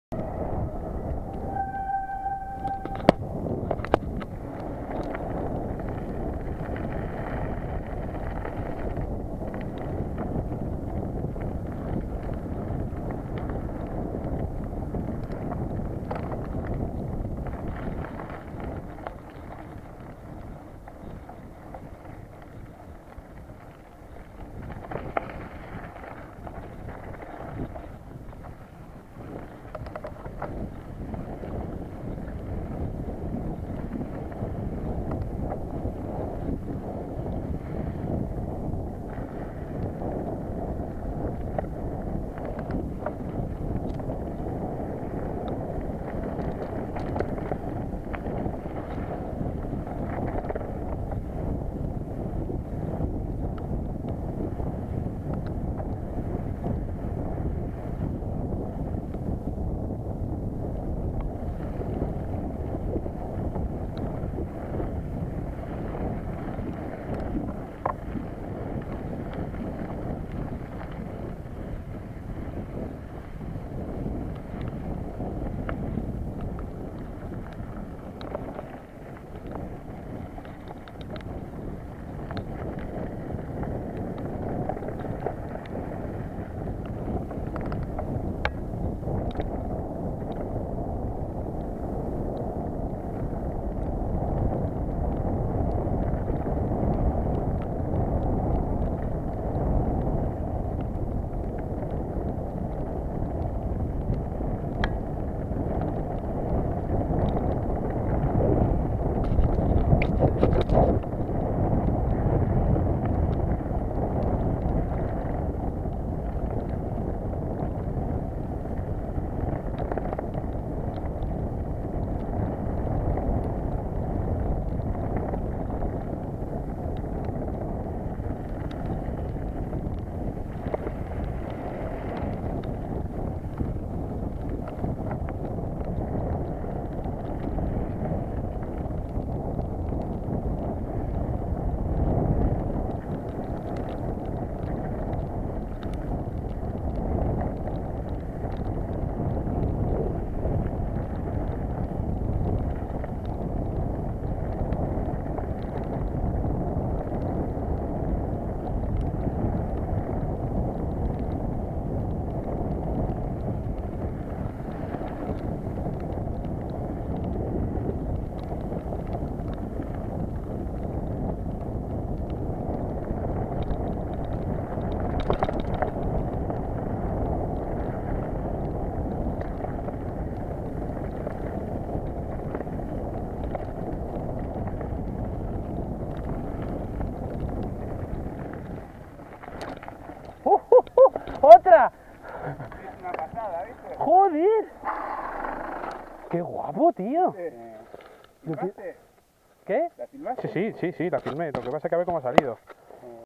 Bajada trialera Montesión
Bajada por la trialera de la urbanización Montesión (Toledo).